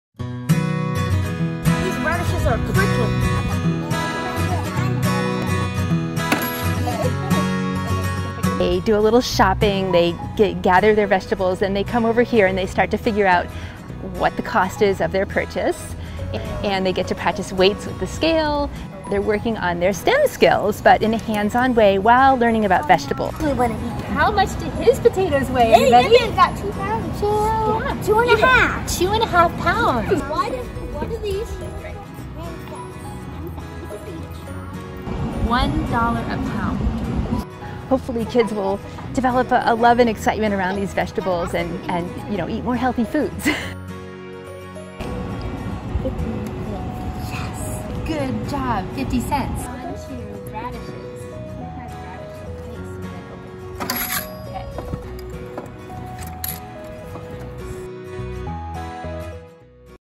Mini Farm Stand Grows Math Skills and Veggie Appeal
James River Elementary third graders shopped for locally grown veggies while putting their math skills to the test. Thank you to WJCC Schools Foundation for funding the mini farm stand, and Mockingbird Farmstead and Williamsburg Community Growers for supplying fresh produce. Watch students grow their math skills and veggie appeal!